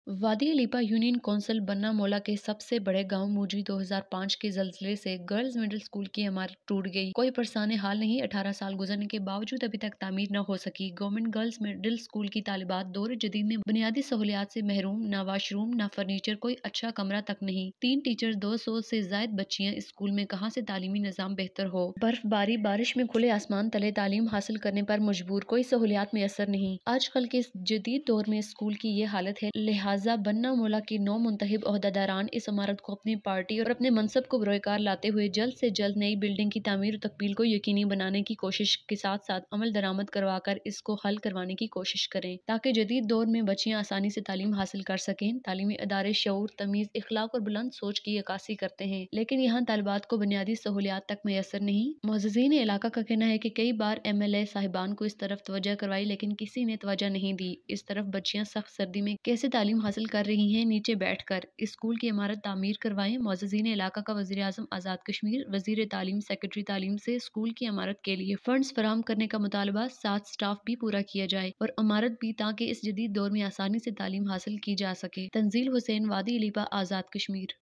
Demand from PM AJK Education Minister Secretary Education to provide funds for school building,
In Muzaffarabad and its suburbs, the snowfall on the upper hills and the rain in the lower areas has stopped for the last two days.